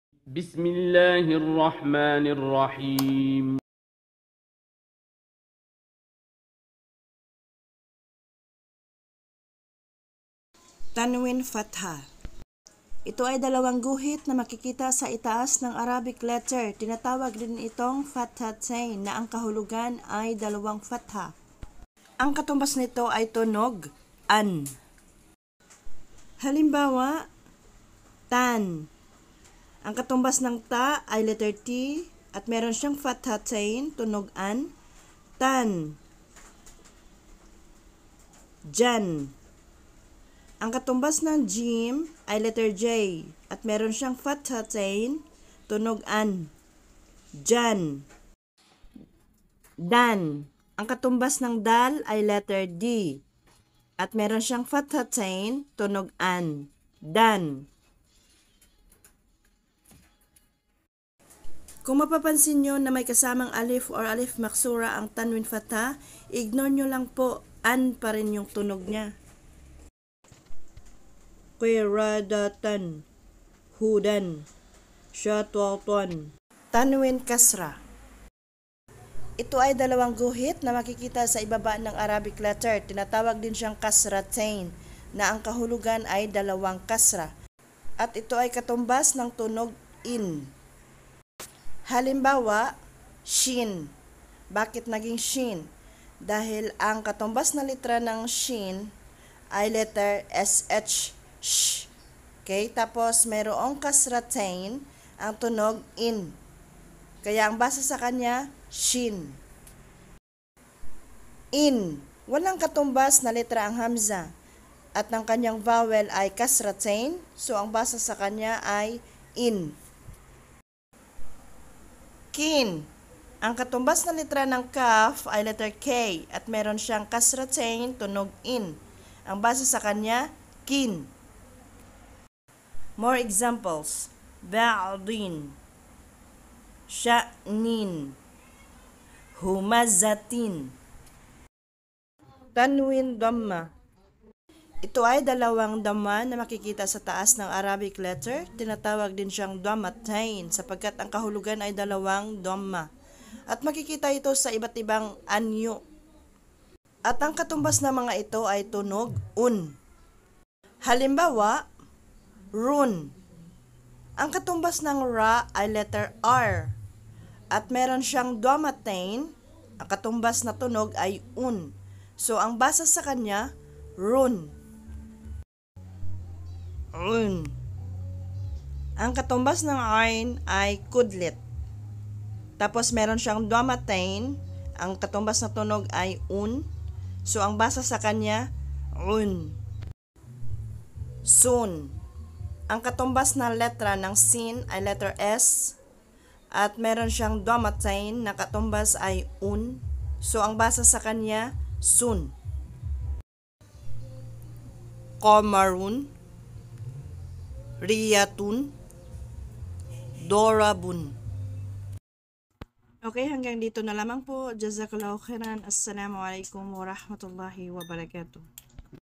Paano basahin ang arabic letter na may Tanween
Tanween fathah, kasrah at dammah. ang tunog nito ng vowel ay 👉🏼 an, in, un